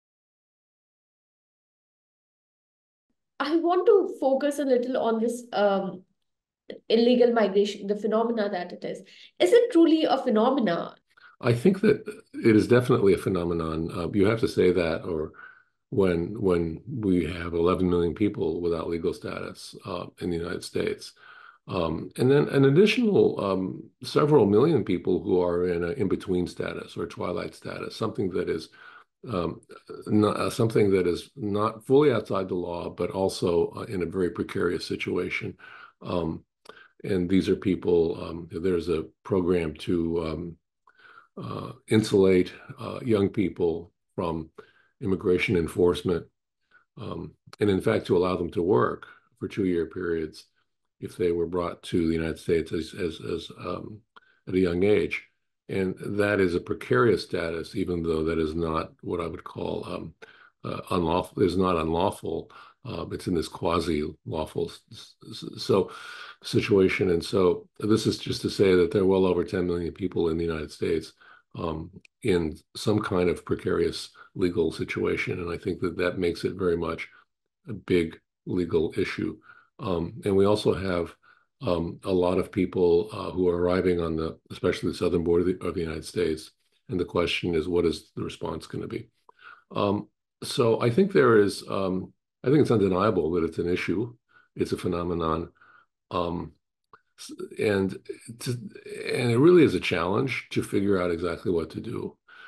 Is Illegal Immigration a Real Problem in the US?
IGPPExpertTalks Special Series on the US Presidential Election, 2024!

IMMIGRATION & CITIZENSHIP IN THE US

Is illegal immigration truly a problem in the US?

To watch the full discussion, visit: youtu.be/s2NeUg_KSXQ